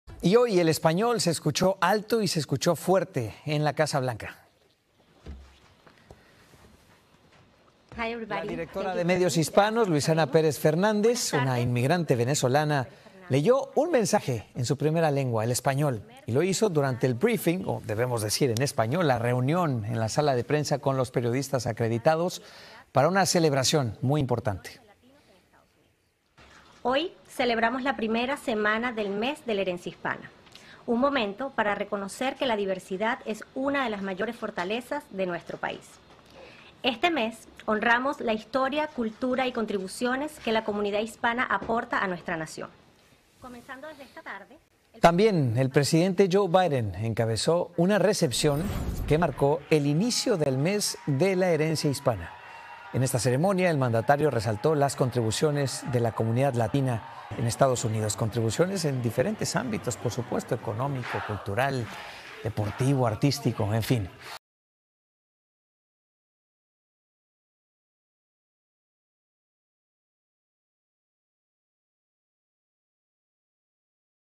En la Casa Blanca también se habla español. Así se escucha en su sala de prensa | Noticias Telemundo
Video oficial de Noticias Telemundo. Como parte de las actividades por el inicio del Mes de la Herencia Hispana, Luisana Pérez, una inmigrante venezolana que es la directora de medios en español de la Casa Blanca, encabezó su rueda de prensa diaria.